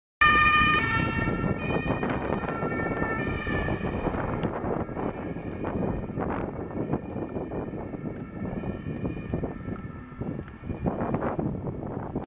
[SEHR SELTEN] Zivilstreife aus Wiesbaden in Gera
Hier zu sehen ist eine sehr seltene Einsatzfahrt von einer Z-Stw (Zivil-Streifenwagen) aus Wiesbaden im April 2017 in Gera.